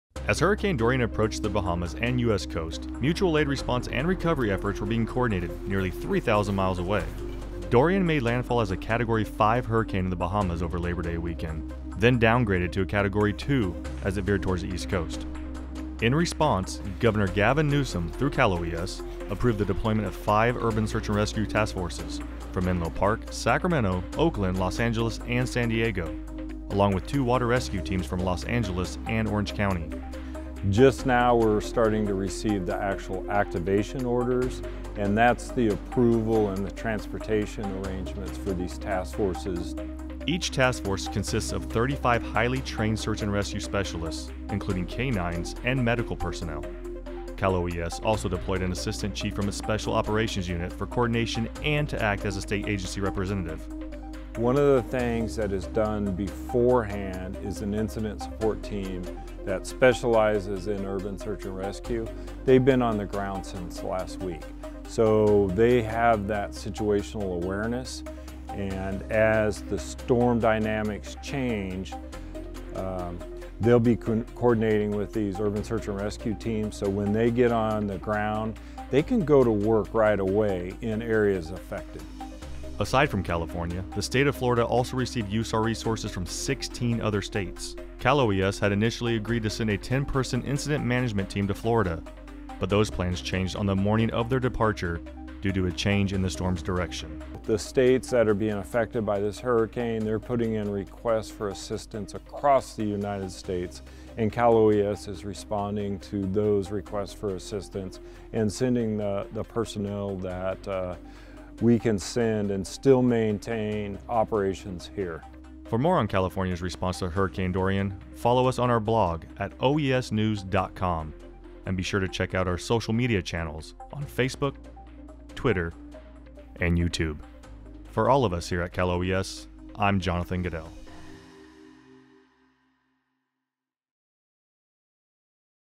NEWS UPDATE: California USAR Teams Deploy to Hurricane Dorian
As Hurricane Dorian veered towards the East Coast, California deployed five Urban Search and Research Task Forces (Menlo Park, Sacramento, Oakland, Los Angeles and San Diego) and two Water Rescue Teams (Los Angeles and Orange County) to assist with response efforts. Each task force consists of 35 highly-trained search and rescue specialists, including K-9s and medical personnel. Cal OES also deployed an assistant chief from its Special Operations Unit for coordination and to act as a state agency representative.